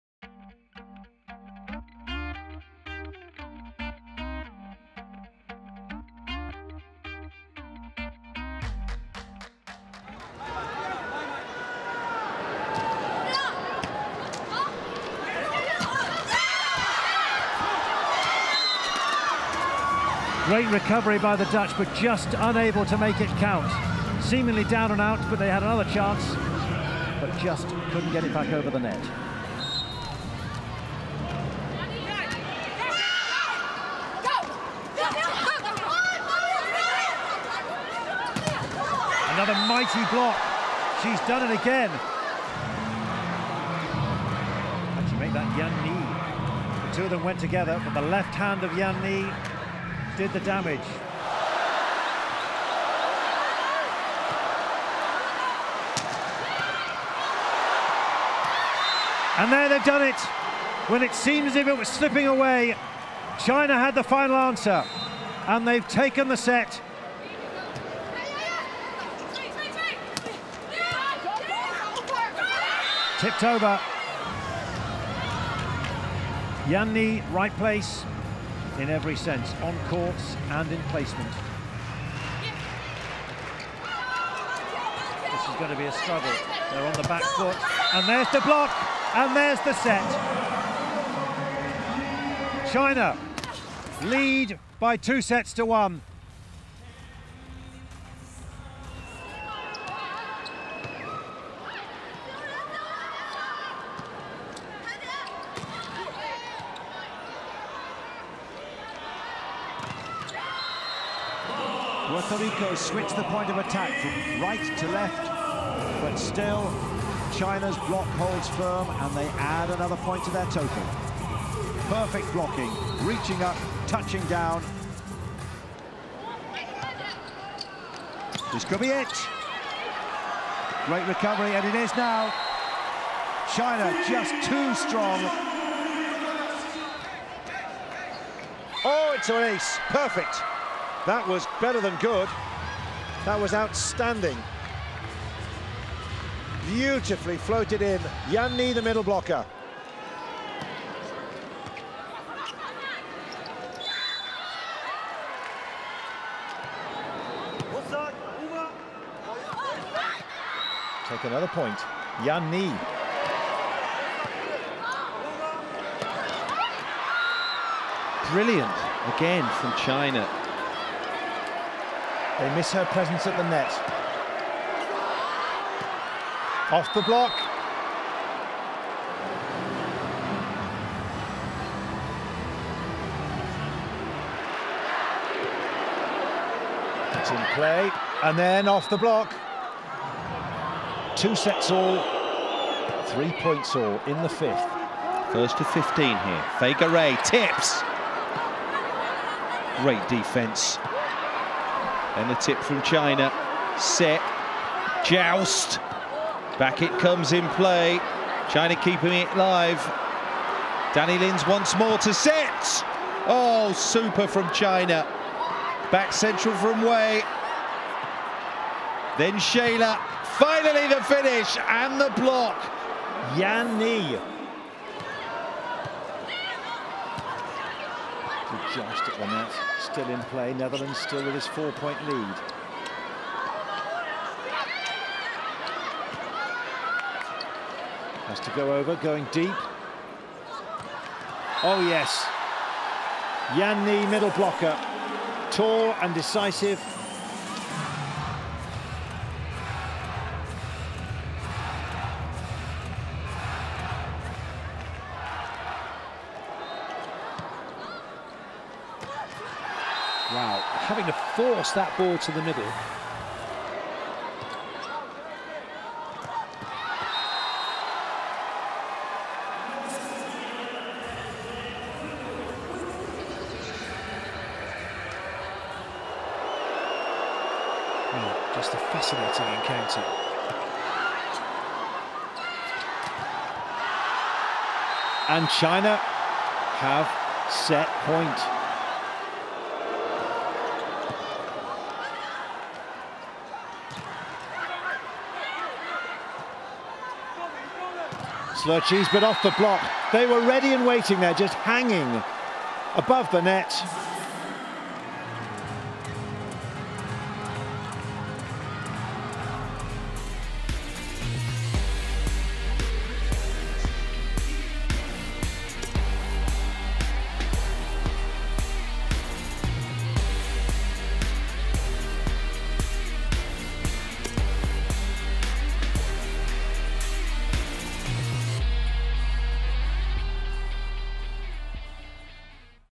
Yan Ni was UNSTOPPABLE at Rio 2016! 🏐
Yan Ni won volleyball gold with team China at the Rio 2016 Olympic Games. They defeated Serbia by 3-1 in the final to take home their 3rd title.

We bring you Yan Ni's best points from Rio. Enjoy watching!